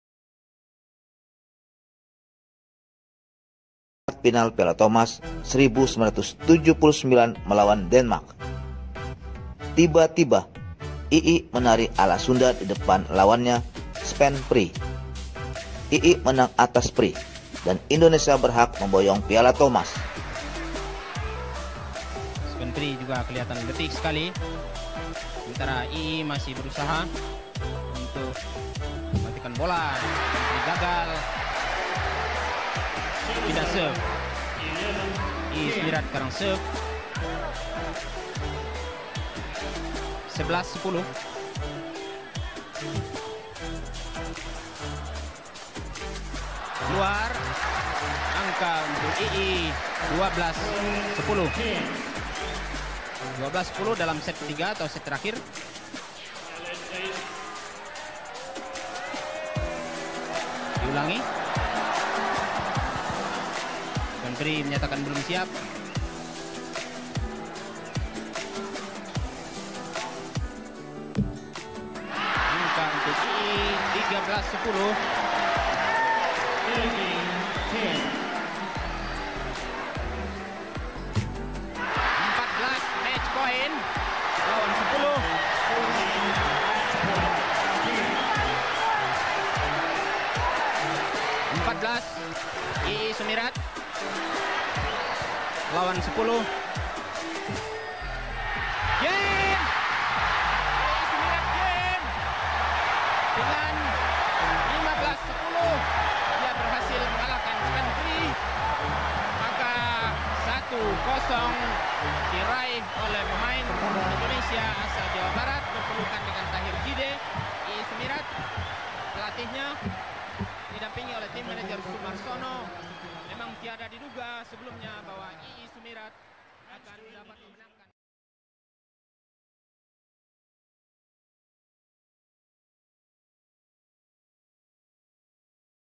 Sang Pembuhuh Naga - Iie Sumirat
Kelahiran Bandung 15.Nov.1950, Iie Sumirat lbh dikenal sbg pemain bulutangkis yg humoris dan eksentrik baik didalam maupun diluar lapangan, salah satunya adalah tarian ala.sundanya didepan lawan (Svend Pri - Denmark) pada pertandingan Final Piala Thomas 1979.
   Sang Pembunuh Naga diberikan sbg julukan Iie Sumirat dikarenakan mampu menghancurkan 2raksasa bulutangkis dunia (Tang Hsieng Hu dan Huo Jiachang) asal China berturut" pada kejuaraan Semifinal dan Final World Badminton Championship di Bangkok tahun 1976.